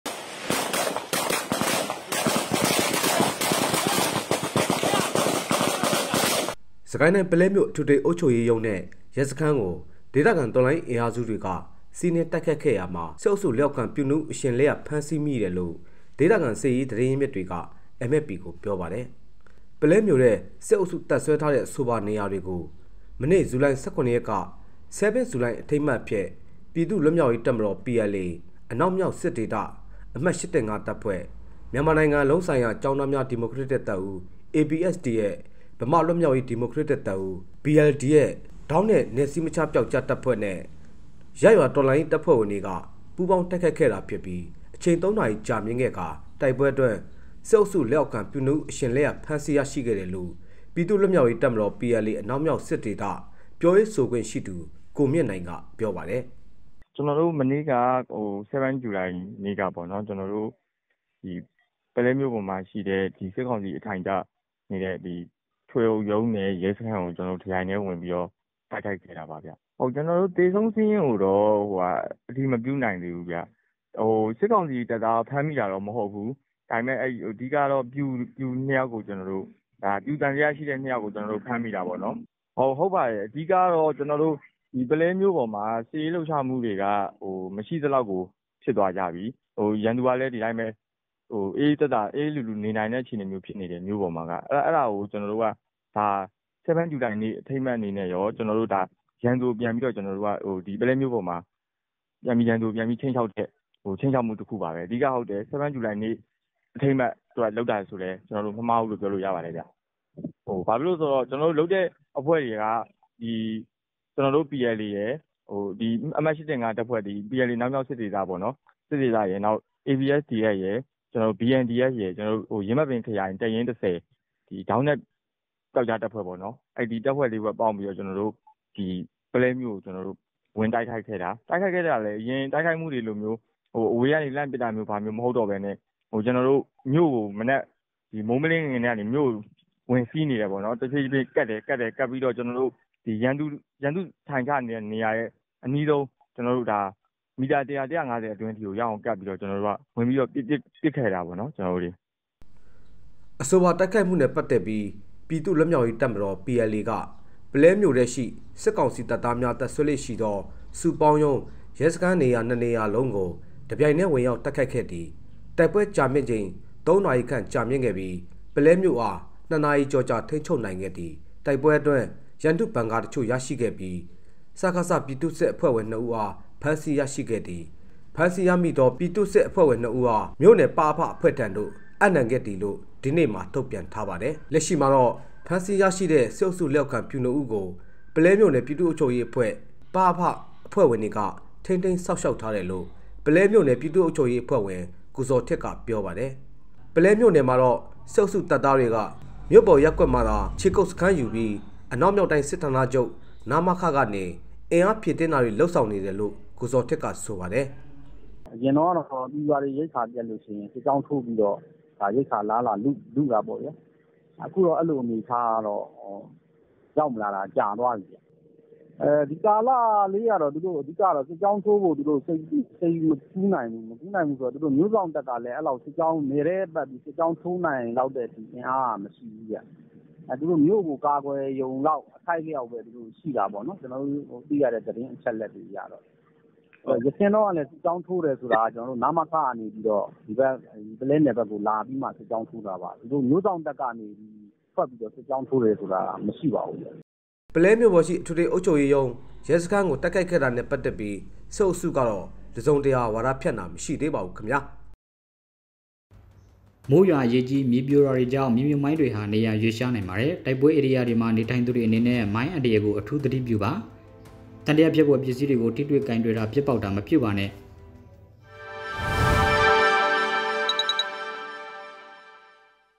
ဆဲဗင်းဇူလိုင် အထိမ်းအမှတ် ပုလဲမြို့ကို စီးနင်းတိုက်ခိုက်၊
မြို့ကို ယာယီ ထိန်းချုပ်နိုင်ခဲ့ပြီး စစ်အုပ်စု ပျူနှစ်ဦး ဖမ်းမိ၊

ကိုသူ/MFP ၊ ဇူလိုင် - ၈

စစ်ကိုင်းတိုင်း၊ ပုလဲမြို့ အထွေထွေ အုပ်ချုပ်ရေးမှူးရုံးနဲ့ ရဲစခန်းကို ဒေသခံ တော်လှန်ရေး အင်အားစုတွေက စီးနင်း တိုက်ခိုက်ခဲ့ရာမှာ စစ်အုပ်စုလက်အောက်ခံ ပျူနှစ်ဦး အရှင်လက်ရ ဖမ်းဆီးမိတယ်လို့ ဒေသခံစစ်ရေးသတင်းရင်း မြစ်တွေက MFP ကို ပြောပါတယ်။

ပုလဲမြို့ထဲ စစ်အုပ်စု တပ်စွဲထားတဲ့ အဆိုပါနေရာတွေကို မနေ့ ဇူလိုင်လ ၇ ရက်က ဆဲဗင်းဇူလိုင်အထိမ်းအမှတ် အ ဖြစ် ပြည်သူ့လွတ်မြောက်ရေးတပ်မတော်(PLA) ၊ အနောက်မြောက်စစ်ဒေသ-အမှတ်(၈၁၅) တပ်ဖွဲ့၊ မြန်မာနိုင်ငံ လုံးဆိုင်ရာကျောင်းသားများဒီမိုကရက်တစ်တပ်ဦး(ABSDF)၊ ဗမာ့လွတ်မြောက်ရေး ဒီမိုကရက်တစ်တပ်ဦး (BLDF)၊ ဒေါင်းနက်-နယ်စည်းမခြားပြောက်ကျားတပ်ဖွဲ့နဲ့ ရပ်ရွာတော်လှန်ရေးတပ်ဖွဲ့ဝင်တွေက ပူးပေါင်း တိုက်ခိုက်ခဲ့တာ ဖြစ်ပြီး၊ အချိန် သုံးနာရီခန့် ကြာမြင့်ခဲ့ကာ၊ တိုက်ပွဲအတွင်း စစ်အုပ်စု လက်အောက်ခံ ပျူနှစ်ဦးအရှင်လက်ရ ဖမ်းဆီးရရှိ ခဲ့တယ်လို့ ပြည်သူ့လွတ်မြောက်ရေးတပ်မတော် (PLA) ၊အနောက်မြောက်စစ်ဒေသ-ပြောရေးဆိုခွင့်ရှိသူ ကိုမြင့်နိုင် က ပြောပါတယ်။